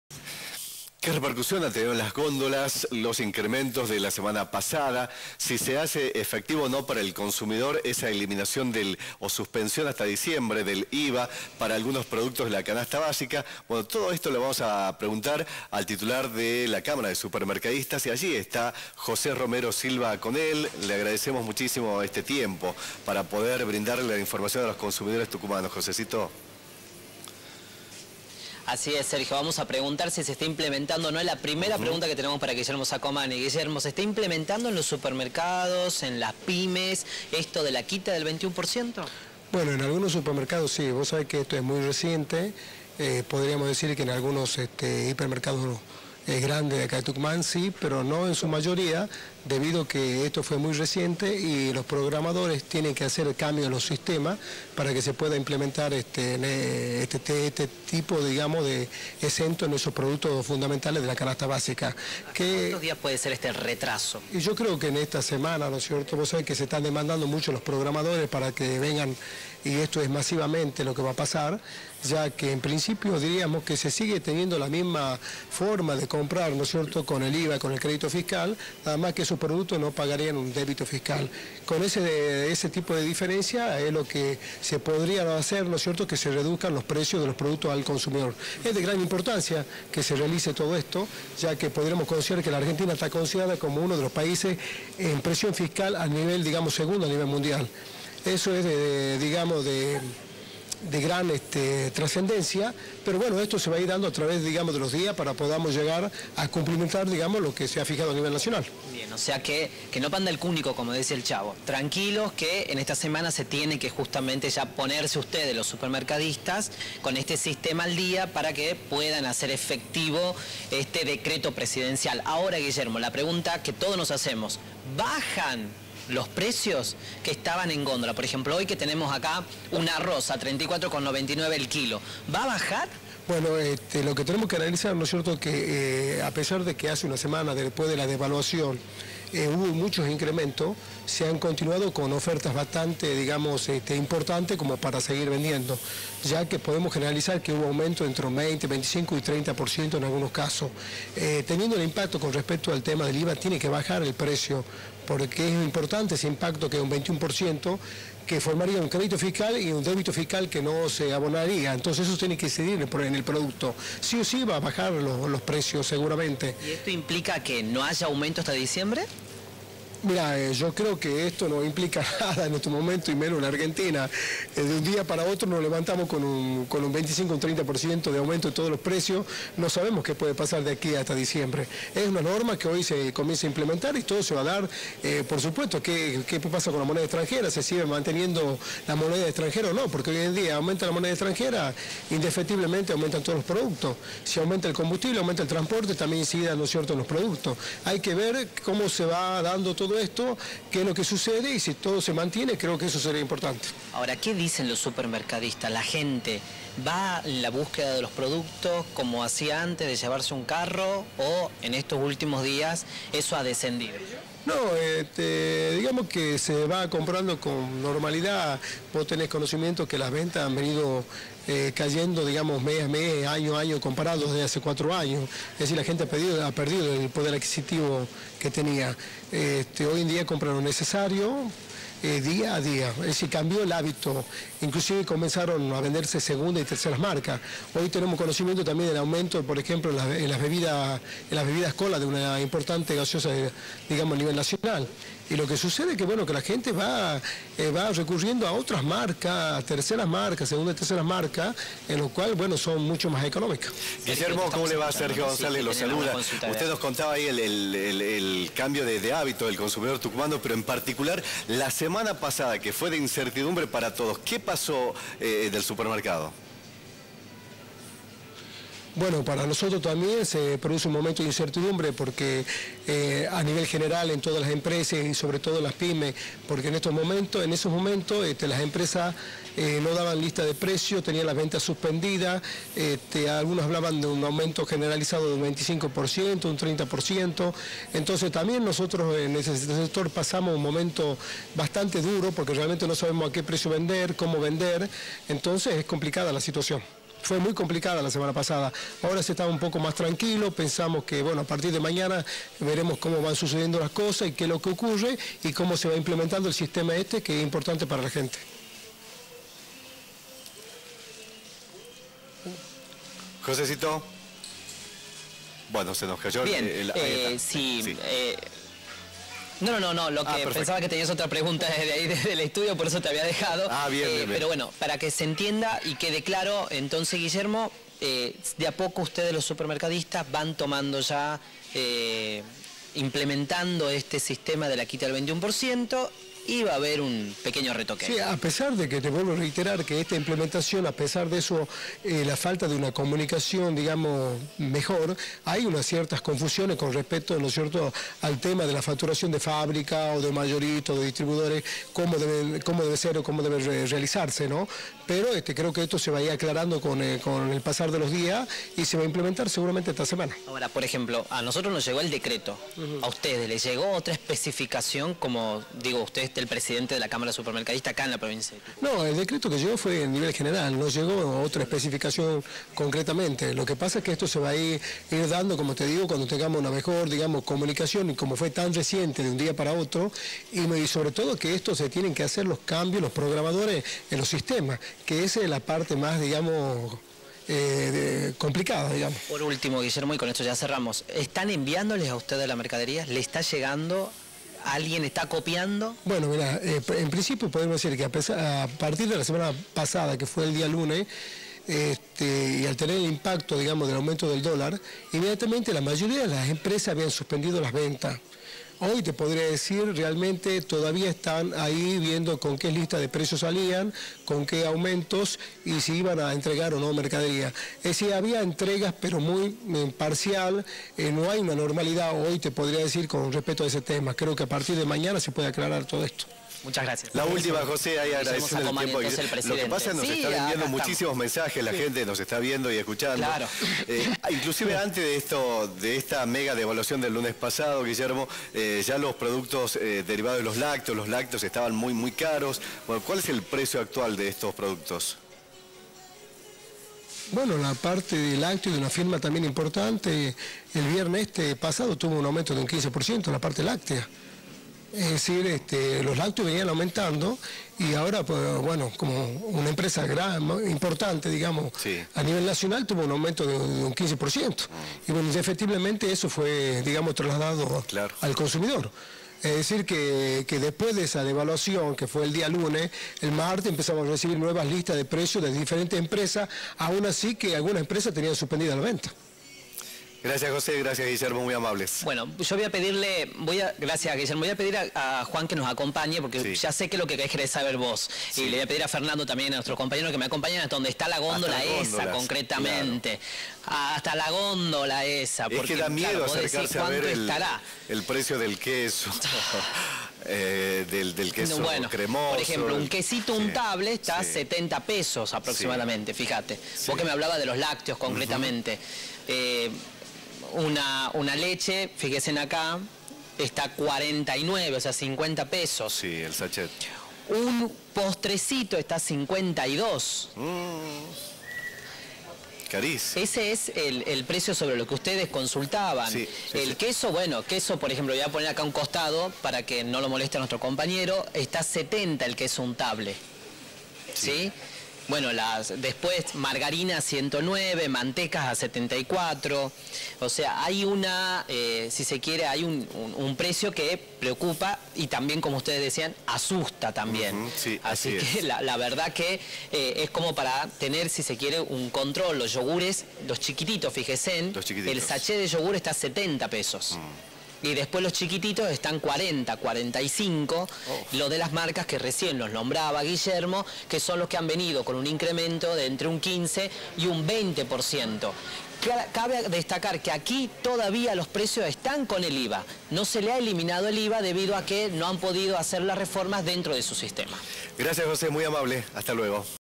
¿Hay productos sin IVA en las góndolas?
Telefe Noticias Tucumán habló con el titular de la cámara de supermercadistas de la provincia.